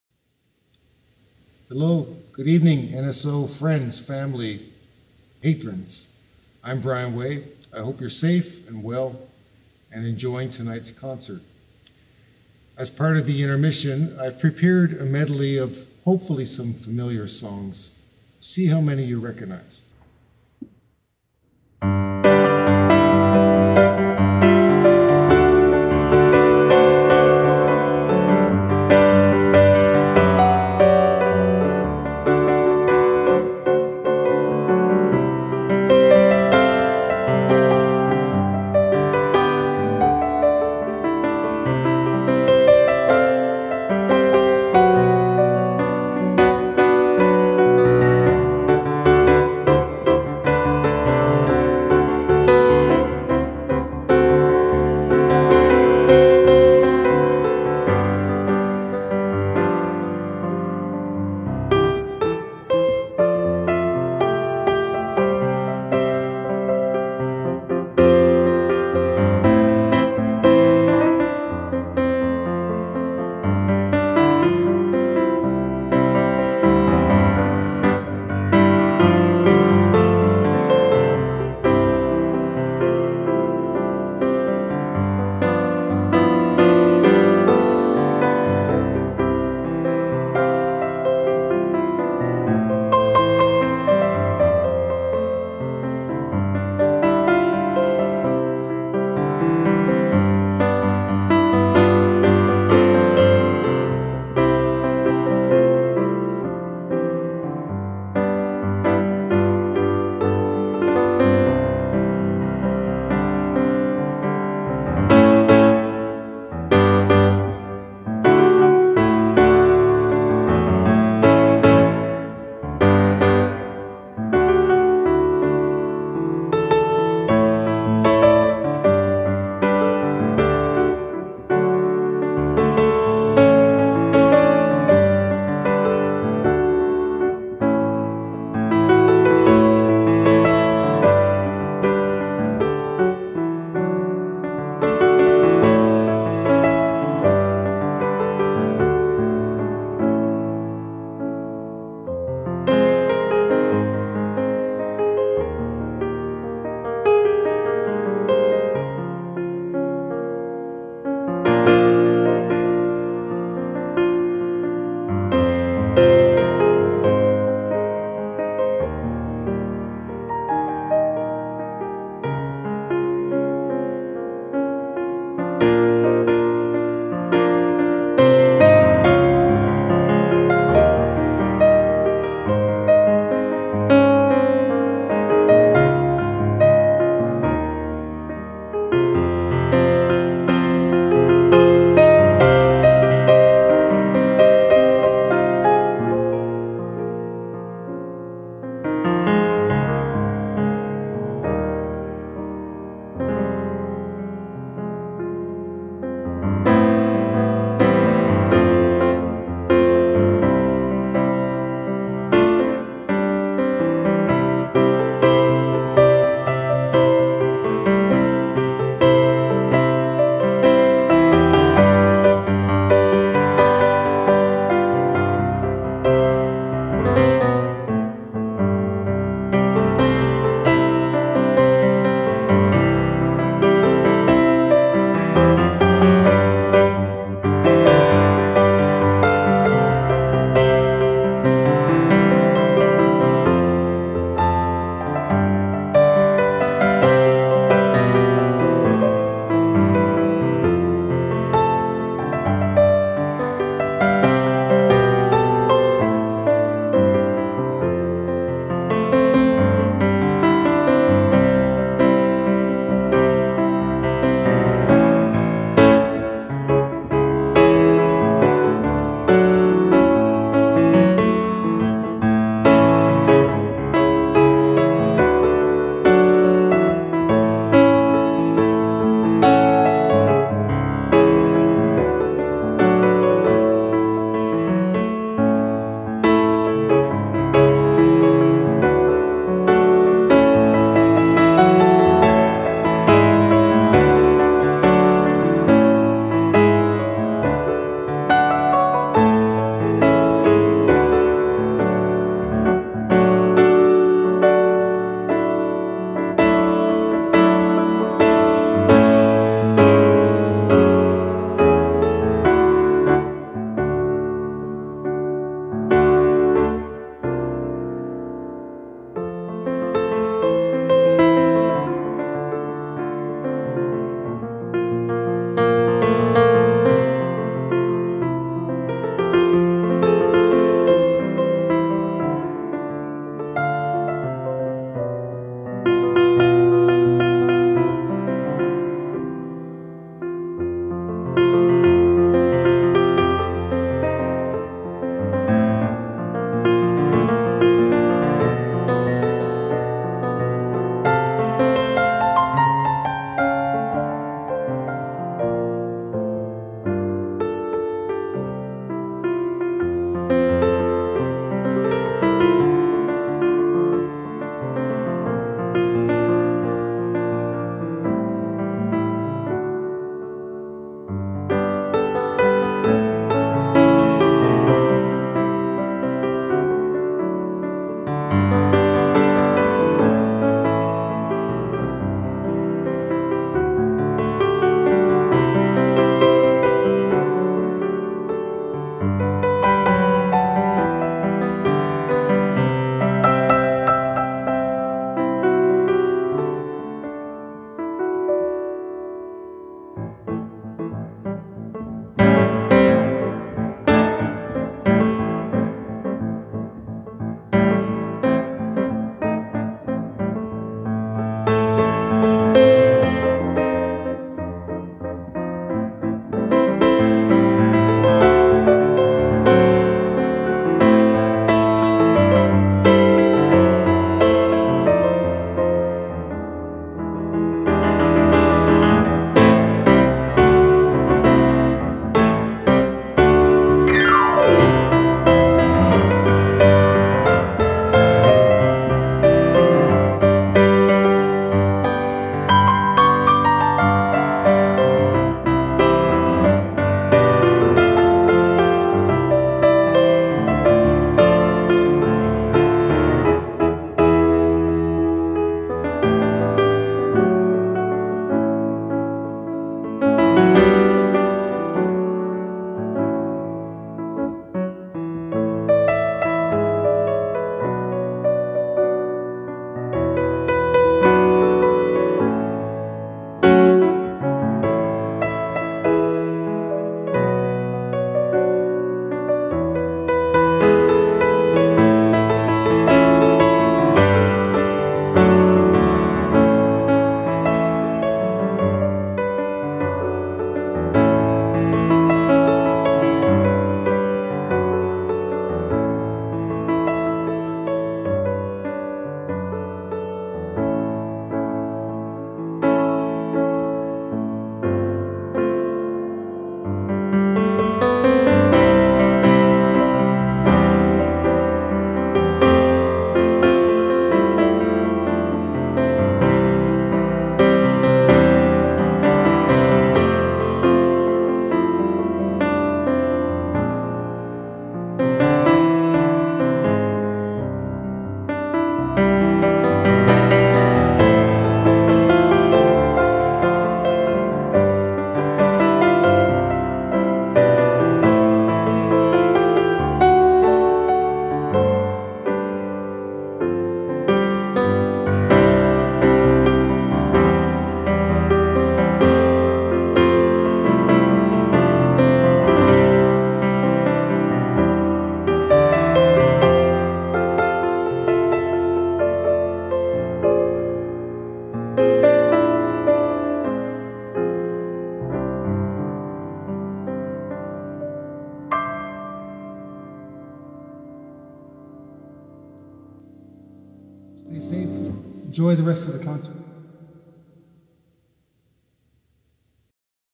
Intermission performance - Newfoundland Symphony Orchestra - Feb 6, 2021
February 6, 2021 - I was delighted to be a guest of the NSO and part of the rhythm section for the 'East Port Properties Big Band Show' presenting the music of the Rolling Stones and featuring my good friend Jodee Richardson.

I was also asked to provide the intermission music. Thank you to the NSO for  permission to post it on my personal social media.

Thanks to my good friend Steve Power for putting this video together!